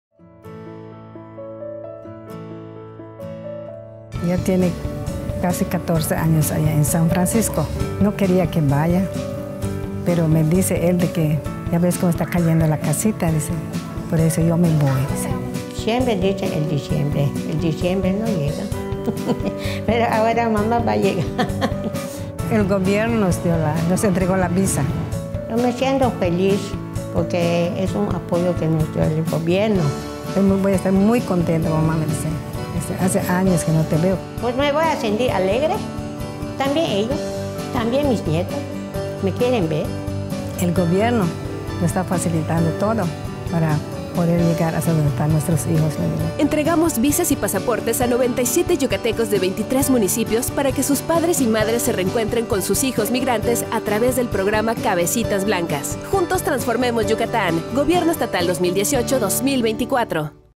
Programa CabecitasBlancas
Con el programa "Cabecitas Blancas" entregamos visas y pasaportes a 97 ciudadanos originarios de 23 municipios de Yucatán, quienes ya se encuentran en Estados Unidos con sus hijos migrantes, disfrutando de su familia después de muchos años de no poder verse.